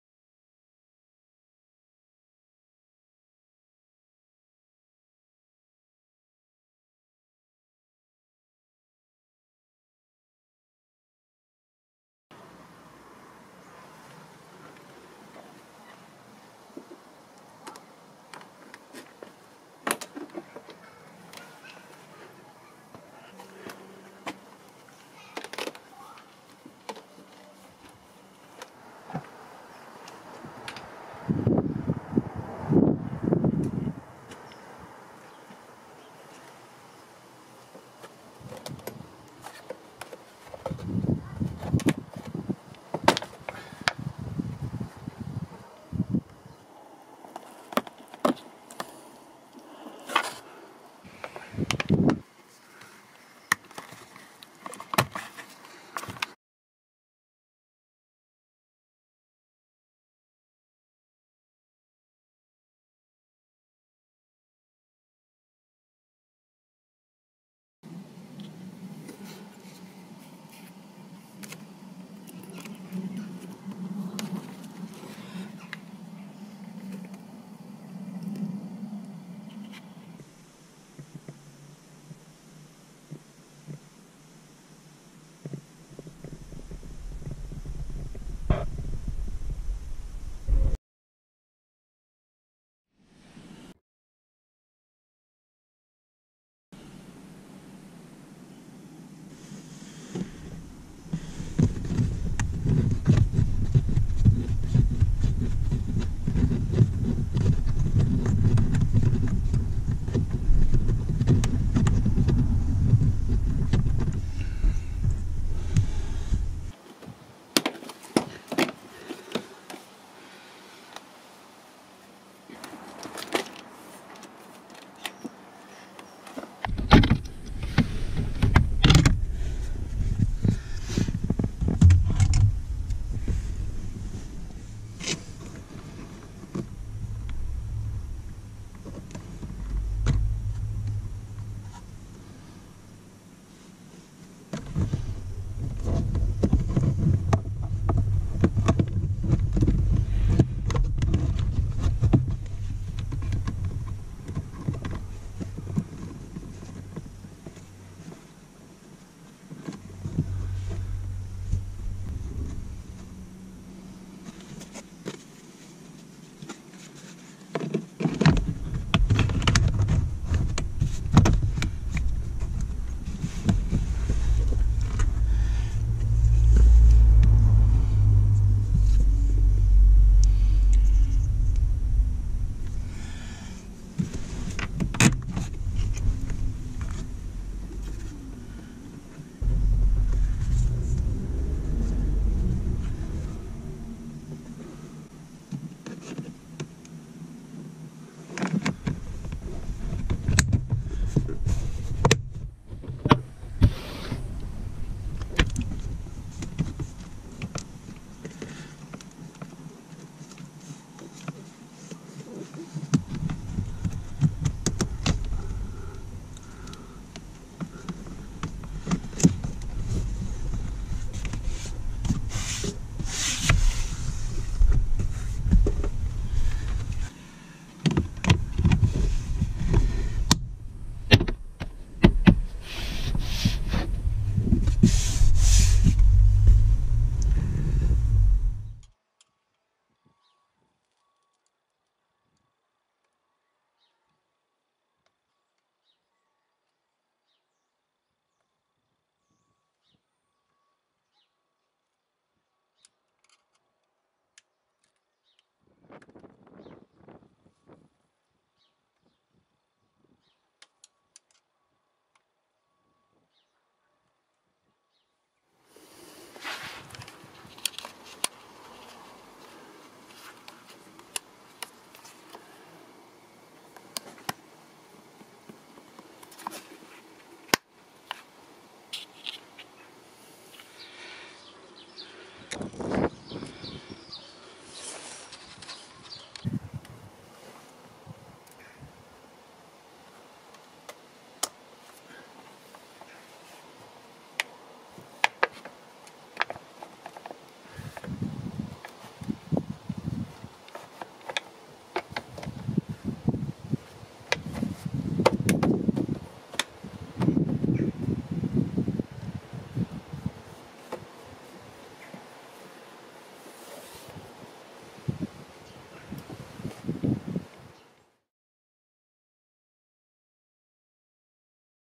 CX-5 and CX-3 A pillar trim
A video tutorial on removing the A Pillar trim and the tether that holds it to the bodywork.  Removing this trim allows easy access to run a wire for a dash cam or other accessory.  Such a wire should not be run around the door seal  and crossed over the airbag as it is dangerous to do so.  The official Mazda method is to cut the tether but it can be re-used as long as it isn’t damaged.  If you choose to renew it, the part number is KB7W-68-162A from any Mazda dealer.  No liability is excepted for following this tutorial - work safely!